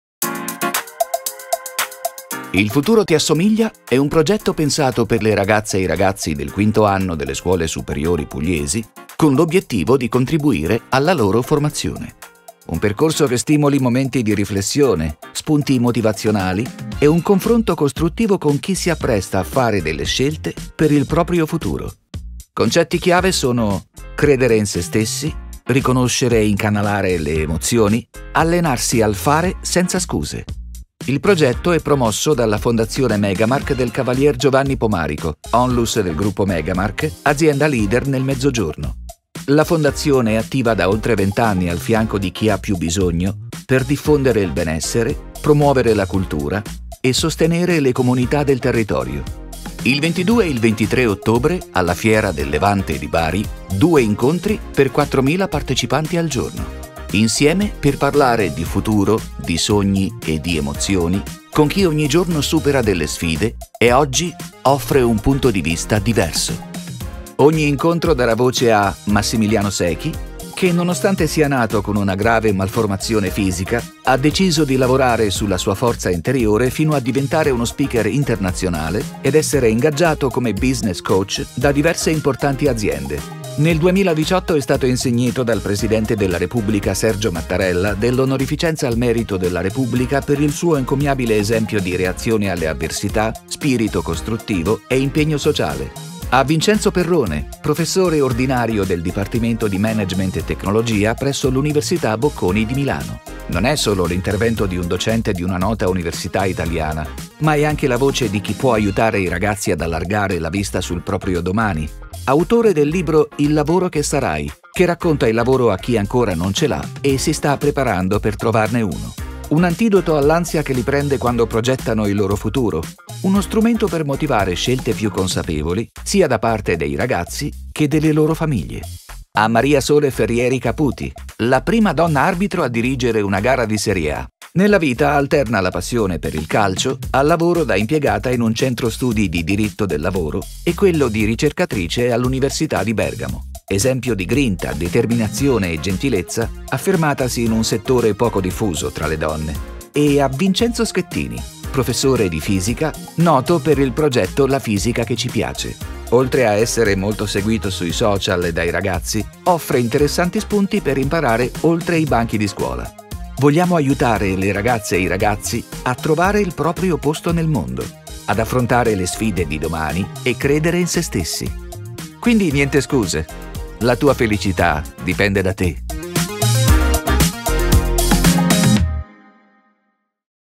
IL FUTURO TI ASSOMIGLIA
Una iniziativa della Fondazione Megamark riservato alle scuole superiori di secondo grado della Puglia e indirizzato ai ragazzi e alle ragazze delle classi IV e V superiore.